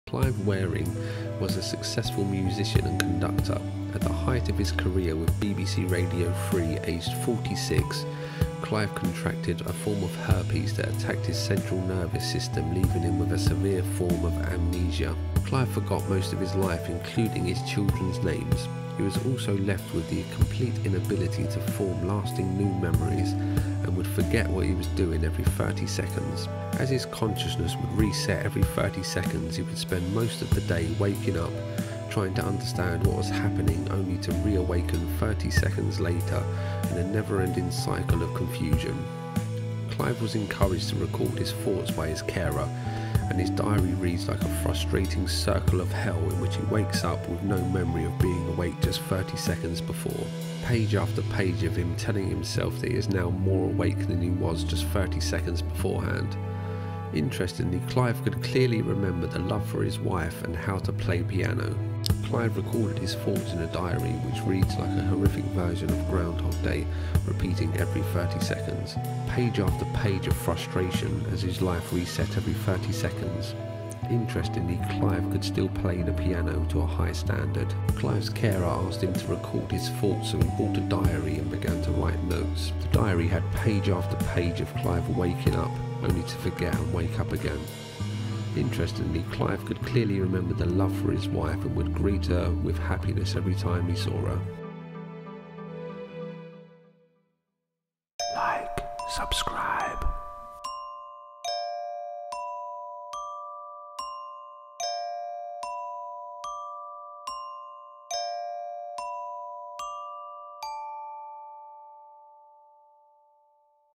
The Man who woke up Every 30 Seconds - Clive Wearing
Clive Wearing was a successful musician and conductor, at the height of his career with BBC Radio 3 aged 46 Clive contracted a form of herpes that attacked his Central nervous system leaving him with a  severe form of amnesia. Clive forgot most of his life, including his children’s names. He was also left with a complete inability to form lasting new memories and would forget what he was doing every 30 seconds, as his consciousness would reset every 30 seconds he would spent most of every day waking up trying to understand what was happening only to reawaken 30 seconds later in a never ending cycle of confusion. Clive was encouraged to record his thoughts by his carer and his diary reads like a frustrating circle of hell in which he wakes up with no memory of being awake just 30 seconds before. Page after page of him telling himself that he is now more awake than he was just 30 seconds beforehand. Interestingly Clive could clearly remember the love he had for his wife and how to play the piano. this is the most extereme case of memory loss